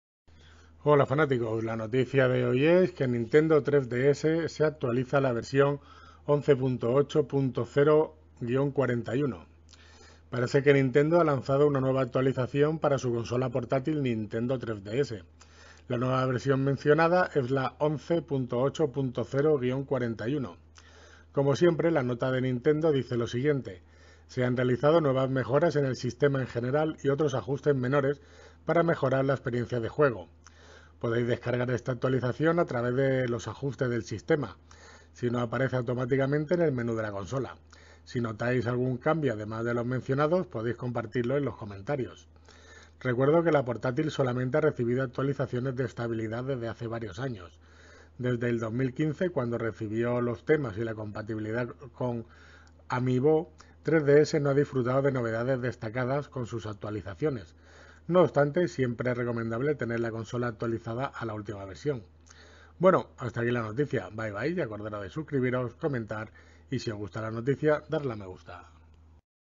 Nintendo 3DS se actualiza a la versión 11.8.0-41. Dannewsgames #667.
Parece que Nintendo ha lanzado una nueva actualización para su consola portátil Nintendo 3DS; la nueva versión mencionada es la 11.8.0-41.

Como siempre, la nota de Nintendo dice lo siguiente:

Se han realizado nuevas mejoras en el sistema en general y otros ajustes menores para mejorar la experiencia de juego.

Podéis descargar esta actualización a través los ajustes del sistema, si no aparece automáticamente en el menú de la consola. Si notáis algún cambio además de los mencionados, podéis compartirlo en los comentarios.

Recuerdo que la portátil solamente ha recibido actualizaciones de estabilidad desde hace varios años. Desde el 2015, cuando recibió los temas y la compatibilidad con amiibo, 3DS no ha disfrutado de novedades destacadas con sus actualizaciones. No obstante, siempre es recomendable tener la consola actualizada a la última versión.

Fuente: Nintenderos.